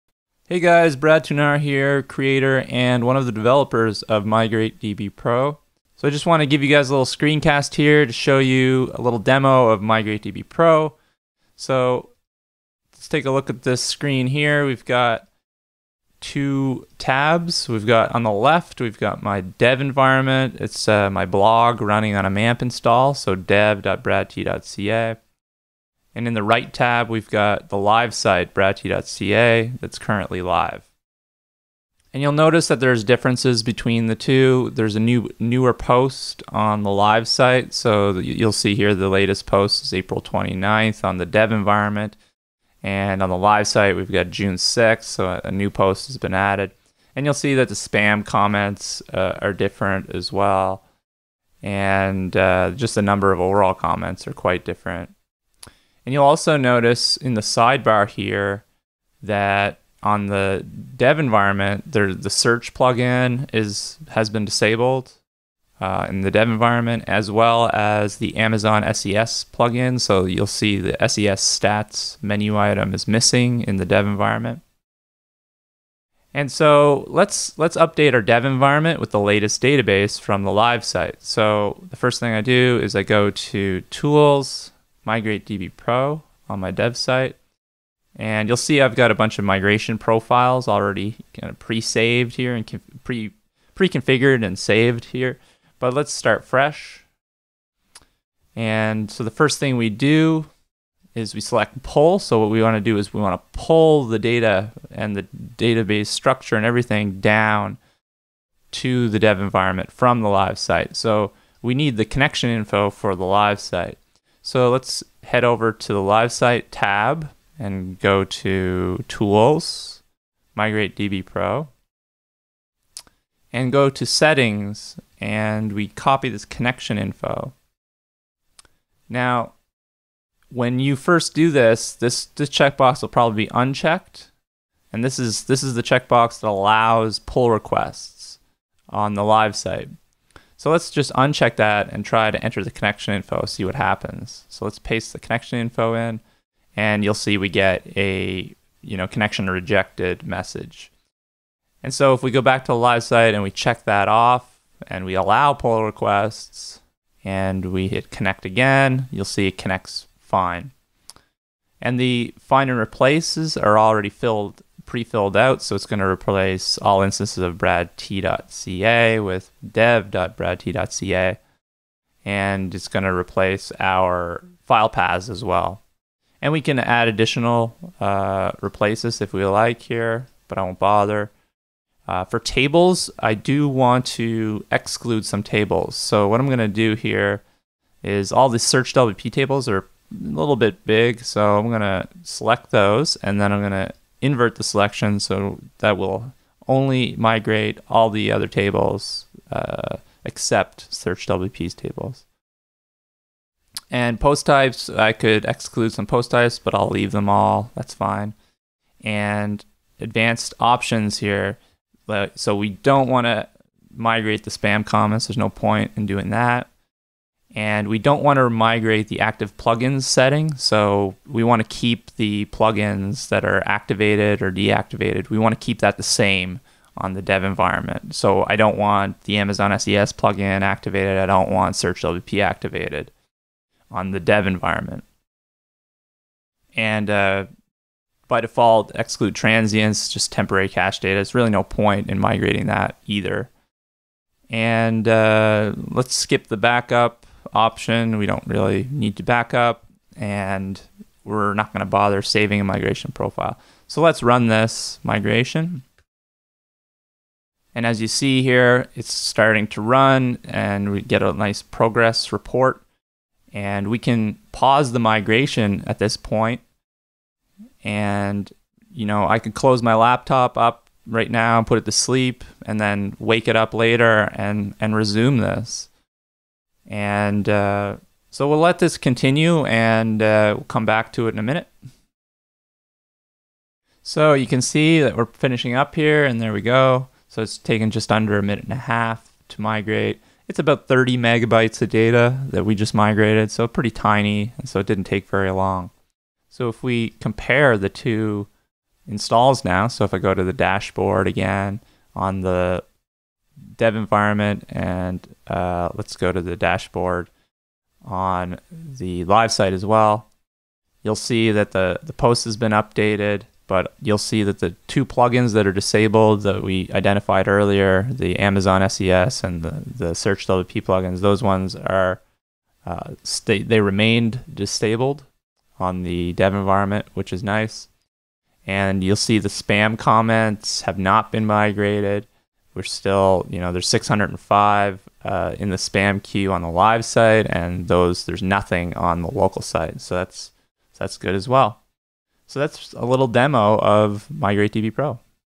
WP Migrate DB Pro - Pulling Live Data Into Your Local Development Environment
This video demonstrates how you can pull data from a remote, live WordPress install and update the data in your local development environment.